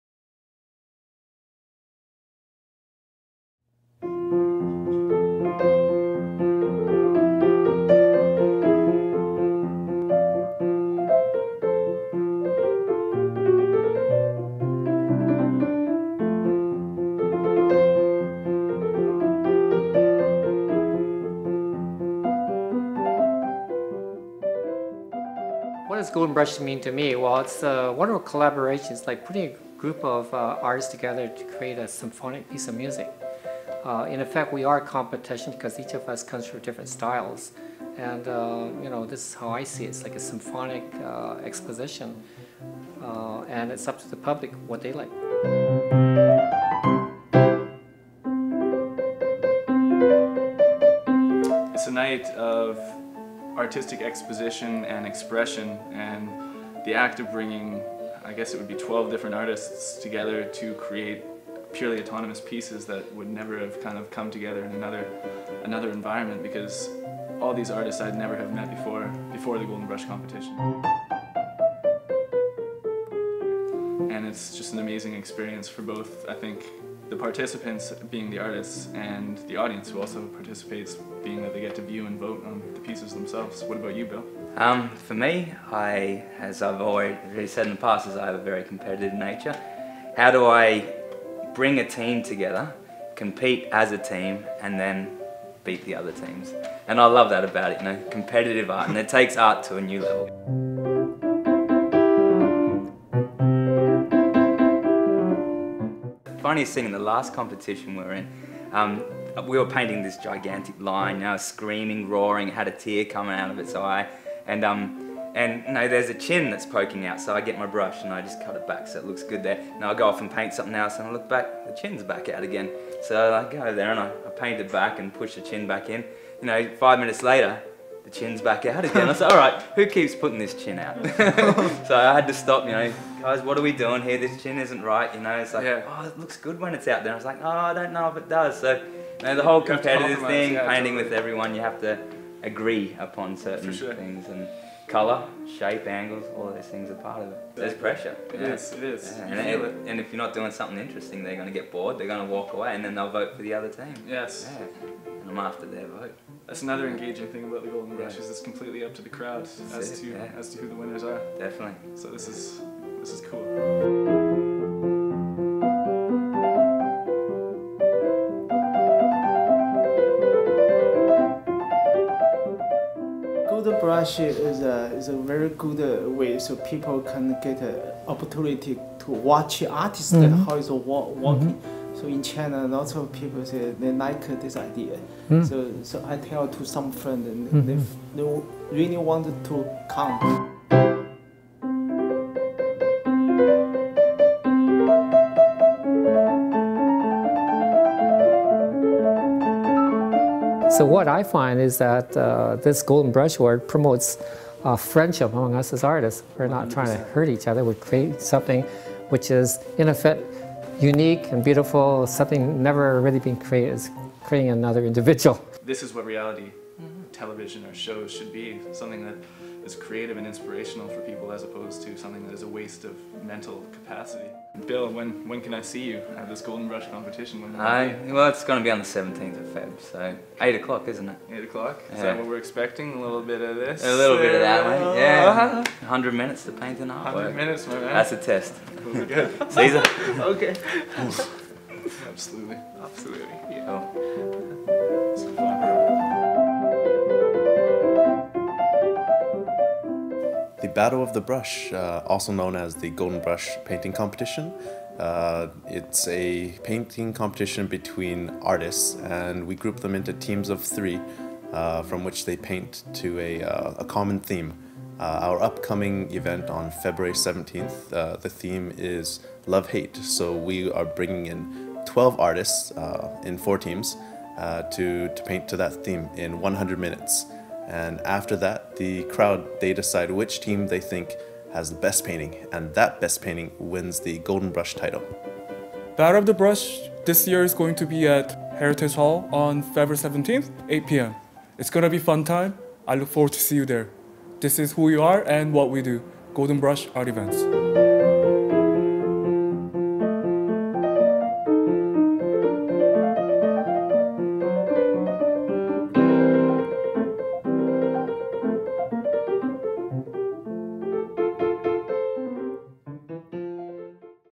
Interview with Battle of the Brush Artists – Preparing for the Grand Finale HD
Battle of the Brush
Interviewing the team captains before they compete in the first Grand Finale of season 1’s Battle of the Brush.

Featuring Raymond Chow, Caesar Hu Jun, David Ullock, and Bill Higginson, these are the captains of the winning teams from season 1.

Artists:
1-Raymond Chow
2-Caesar Hu Jun
3-David Ullock
4-Bill Higginson

Filmed & Edited By:
Jordan Mayfield
of

Produced by:
Kevan Seng-Golden Brush Founder
J Won Suh-Creative Director
Jordan Mayfield-Videographer/Videoeditor